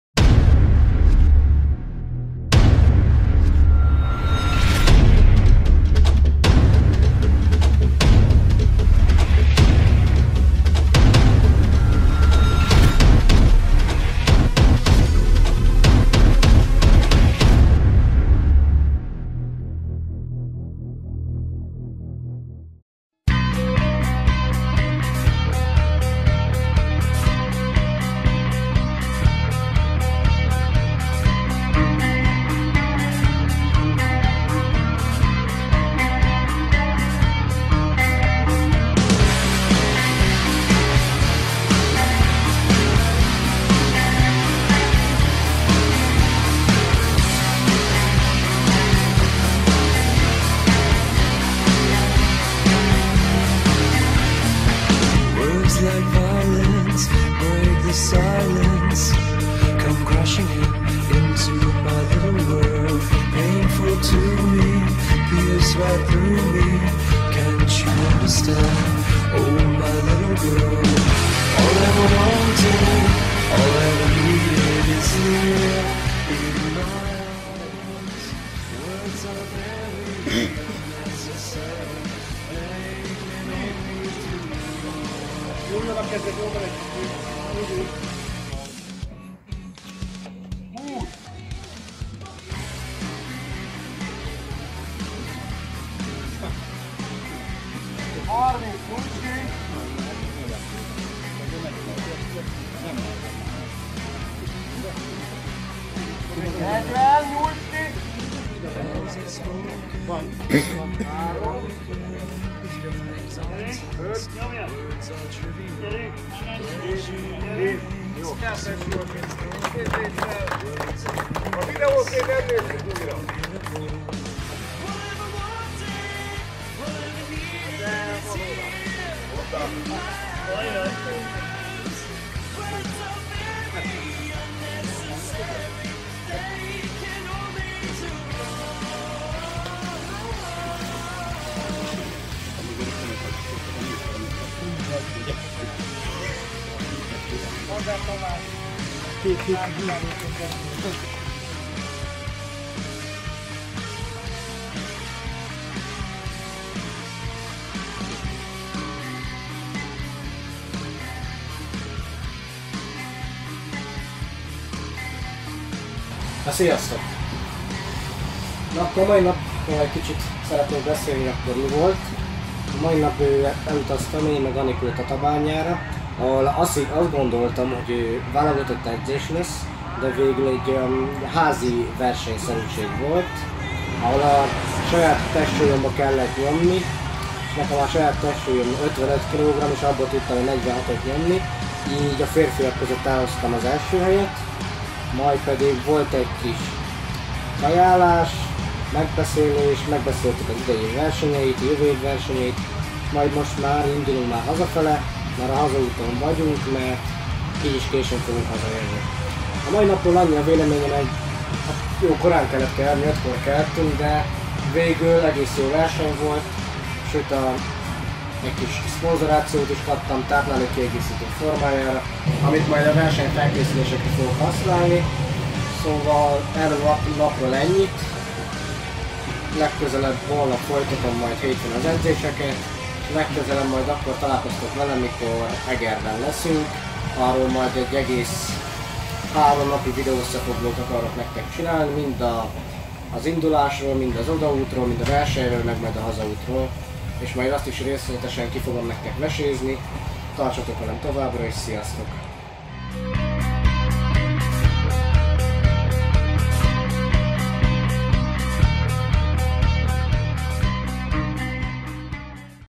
Home competition in Tatabánya
Részt vettünk Tatabányán egy kis csapatépítő "tréningen", ahol jó hangulatban a saját testsúlyomat (55 kilót) 46-szor nyomtam ki és megnyertem ezzel a csatát.

Kaptam egy kis ösztönző csomagot is, amely segít a további fejlődésben,  hálásan köszönöm! :)